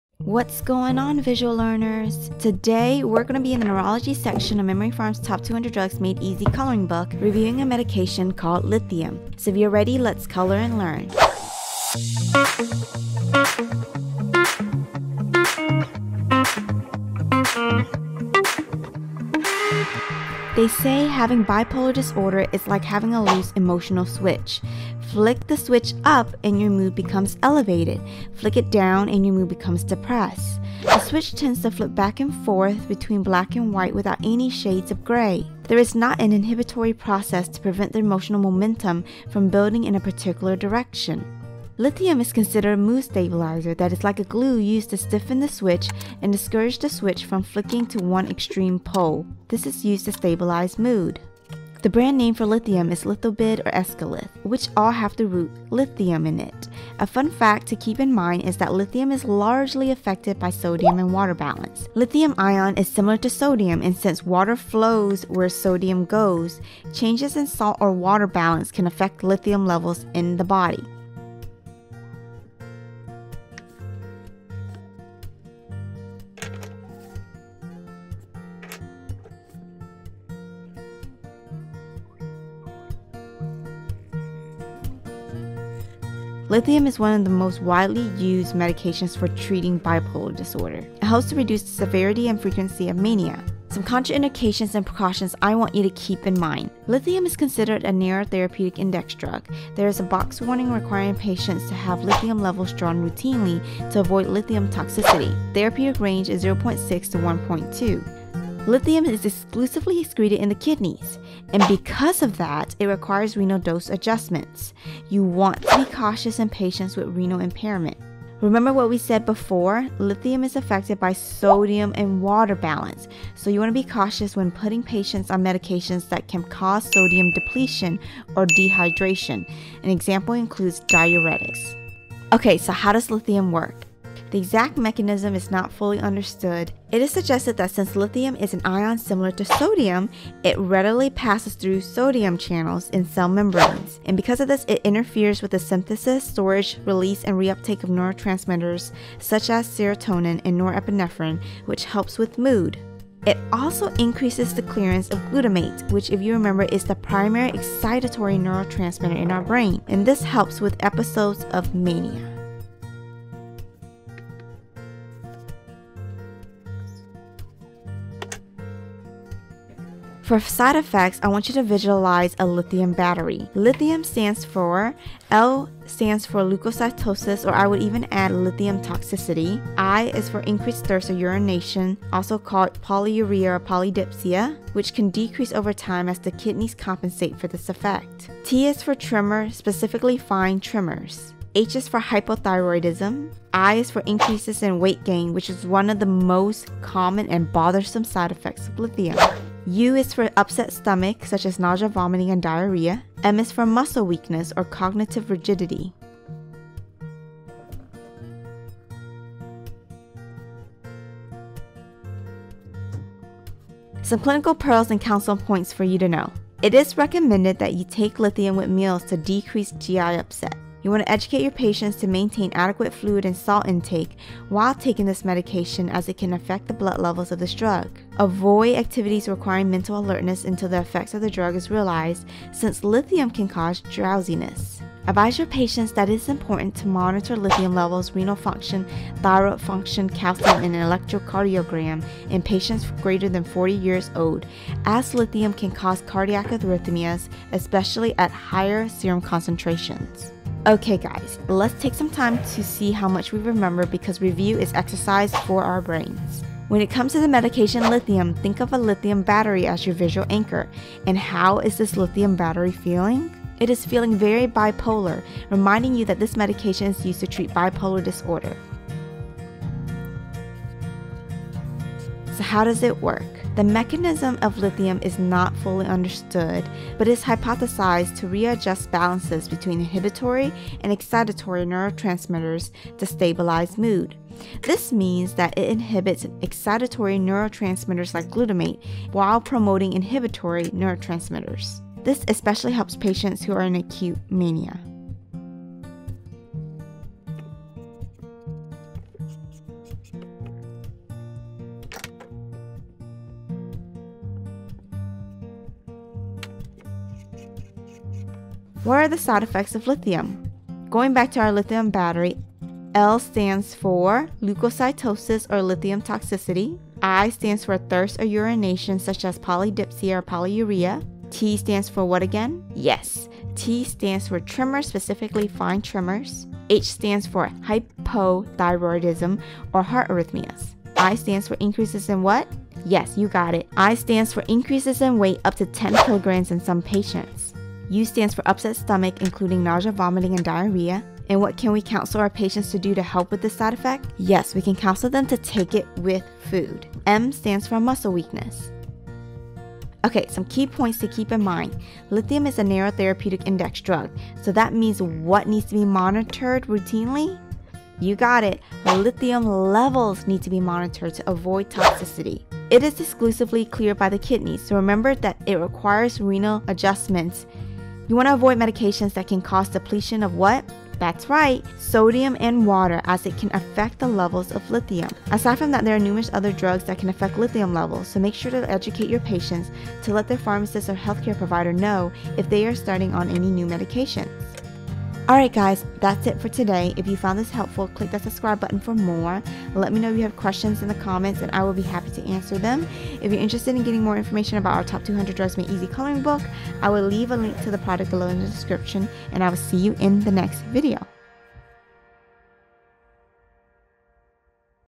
Lithium Made Easy (Mnemonics, Mechanism of Action, Side Effects, Counseling Points)
If you struggle with knowing what key facts to memorize about medications, I am here to help you. This video will be going over the main drug facts about Lithium in a fun and visual way with memory tips and a quick review. It includes drug names, memory tips, indications, side effects, contraindications, mechanisms of action, and counseling points.

Top 200 Drugs Made Easy Coloring Book:

Check out our study resources for mnemonic flashcards, workbooks, and coloring pages on our

Thanks for stopping by! And remember to never stop learning!